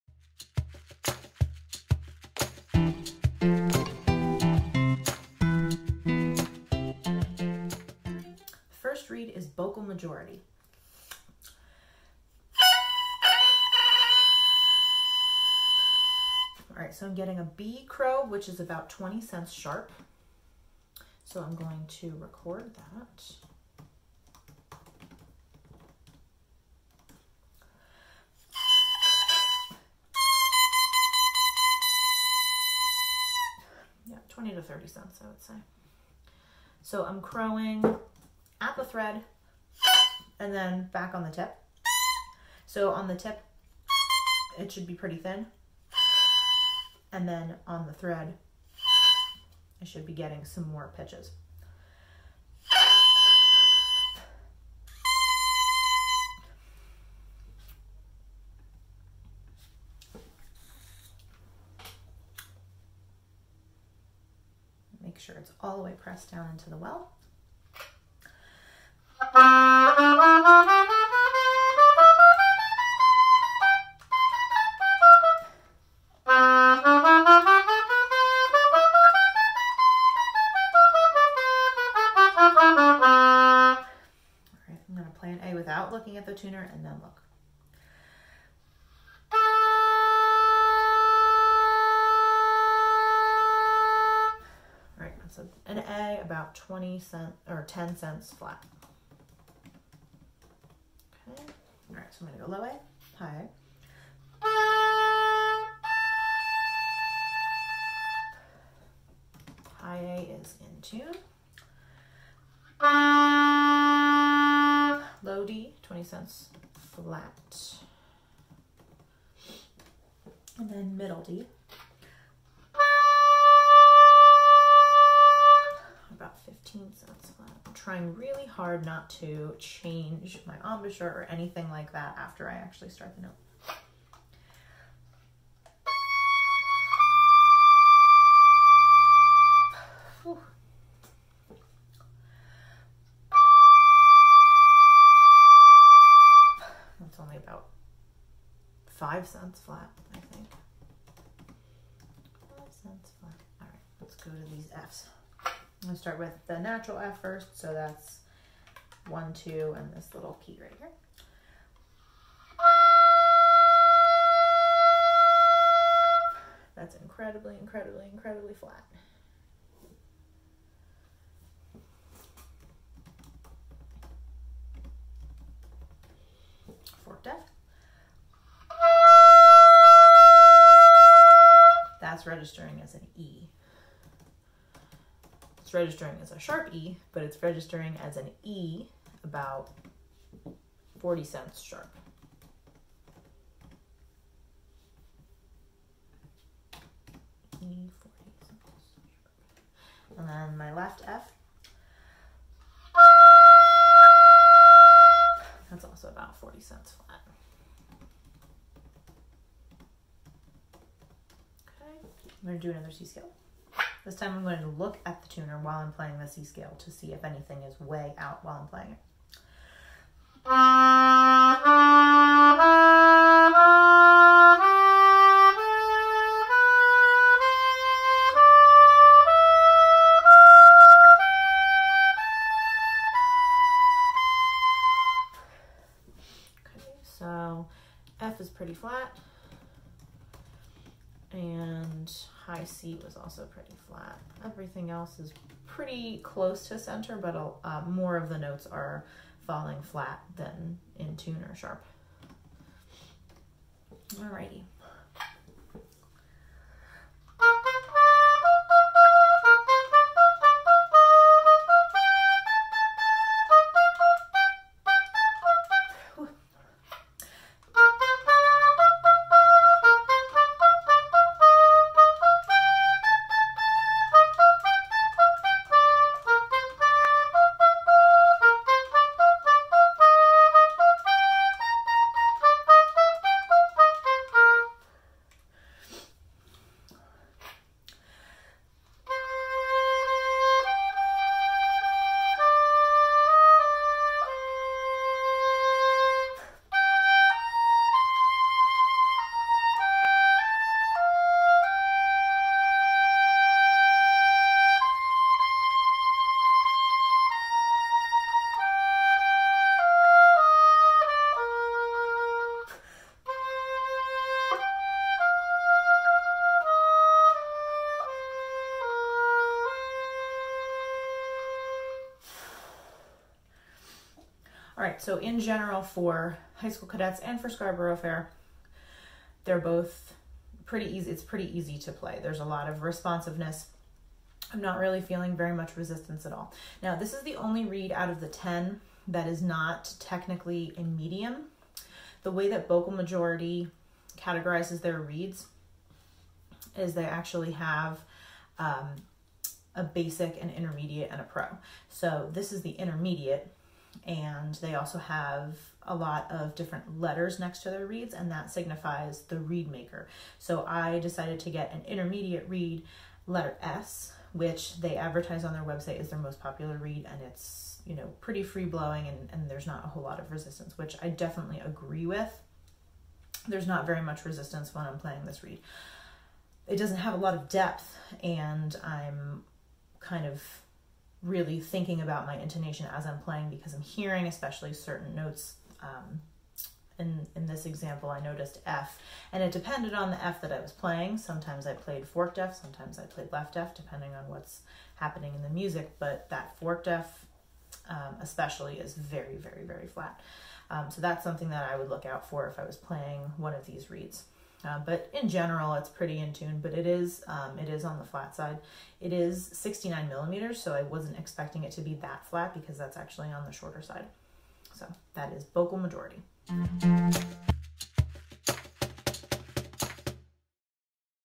Oboe Reed Comparison Series - Bocal Majority
Meg plays and talks about a Bocal Majority oboe reed.
Download the chart comparing all 10 reeds in the series